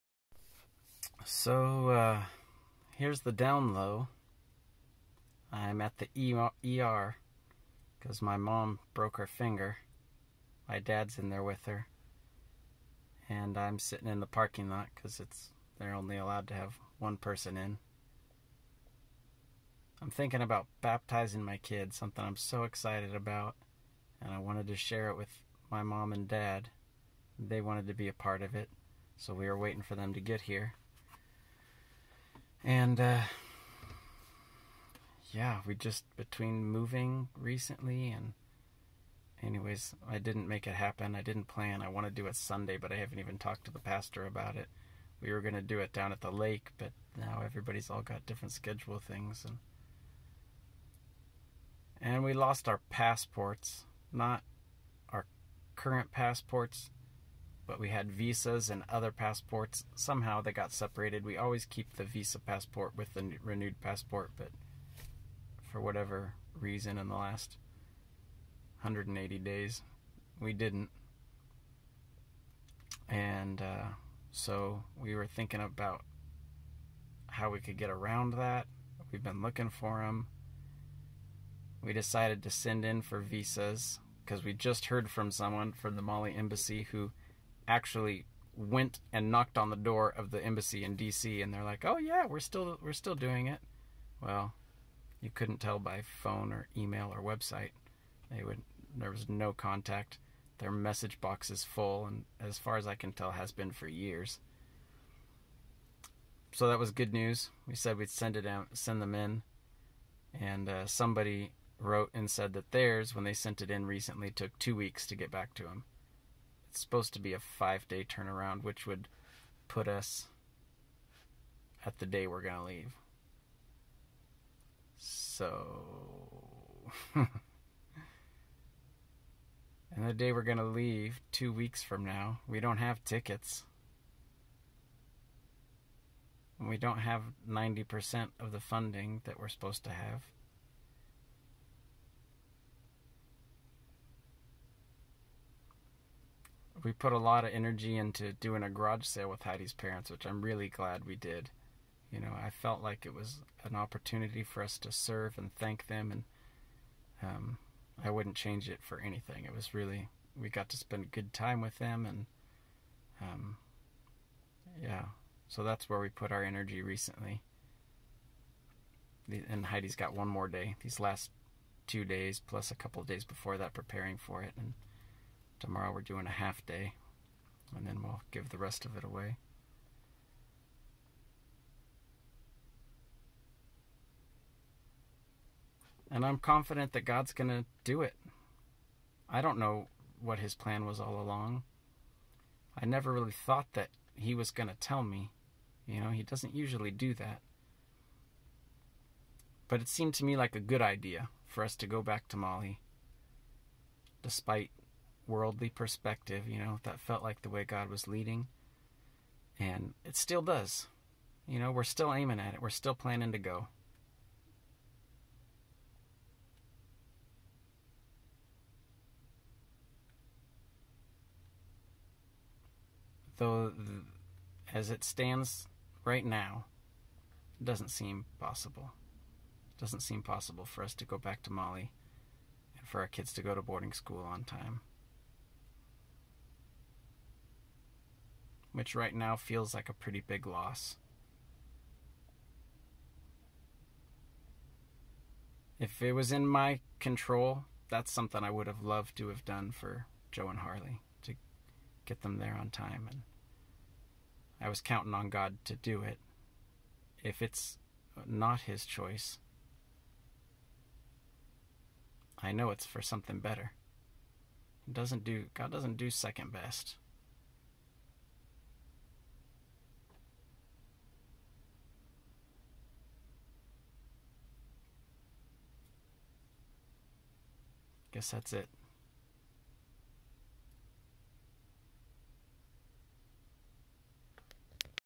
Vlog 2020 August 14 "At the ER."
Taking your mom to the ER and having to wait outside is enough to get you down. I allowed myself a little pity party and examined the worries/fears that threaten to creap in from time to time.
My confidence is really in Christ, so I don't feel irresponsible when things get out of hand...I mean out of my hands. Just watch what He'll do!